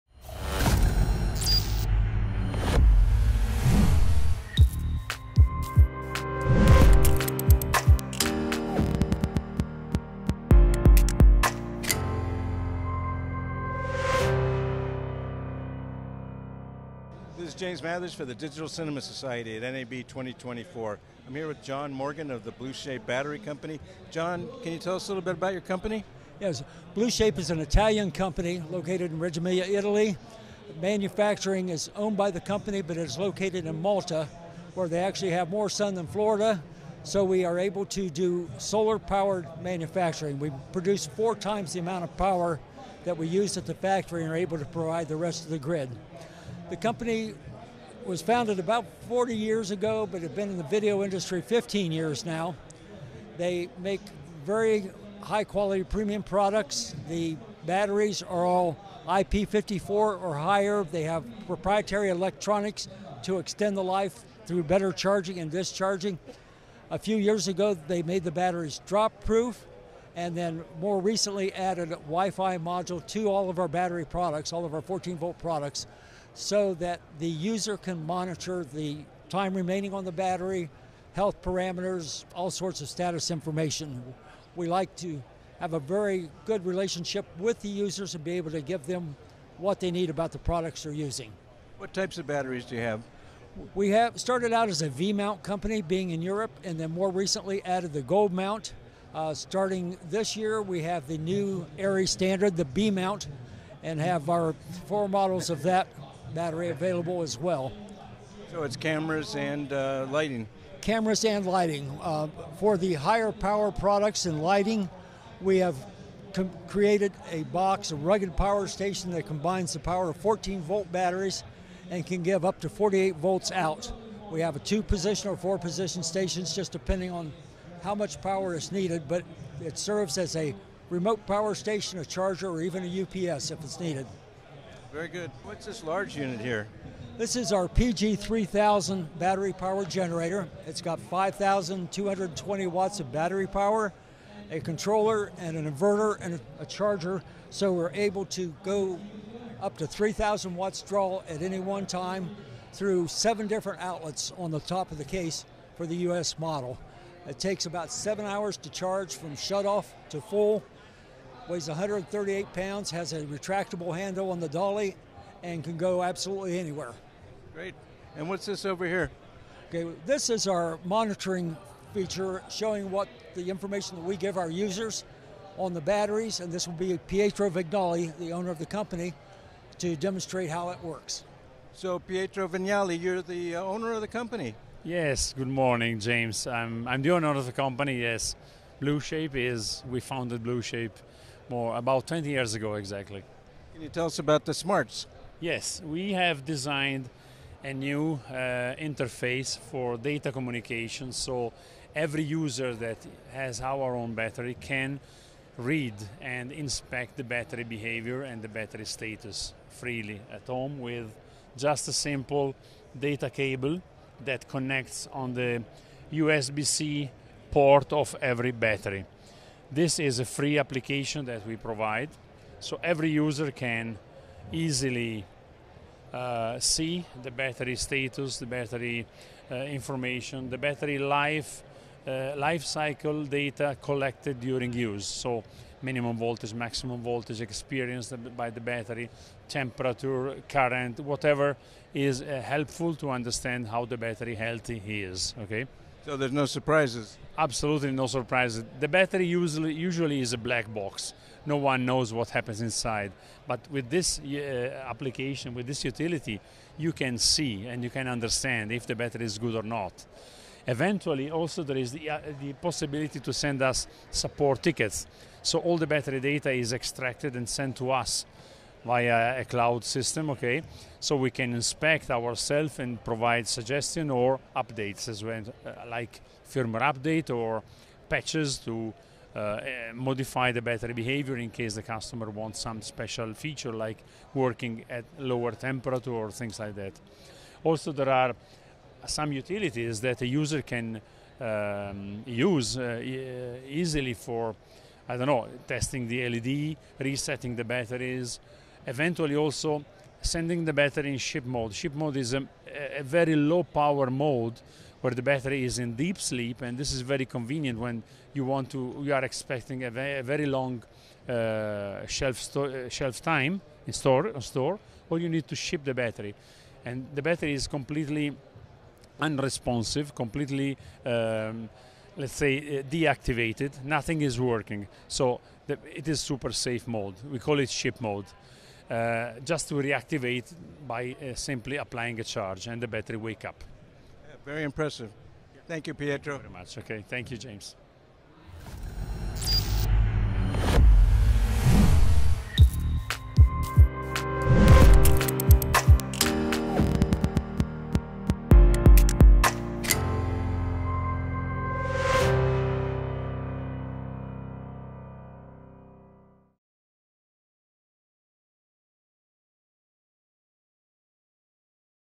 BLUESHAPE - John Morgan - NAB 2024
James Mathers interviews Blueshape’s John Morgan about the company’s history and eco-friendly manufacturing, and their series of batteries, chargers, and generators. They are then joined by Blueshape owner, Pietro Vignali, who demonstrates their SMARTMon real-time battery diagnostic system.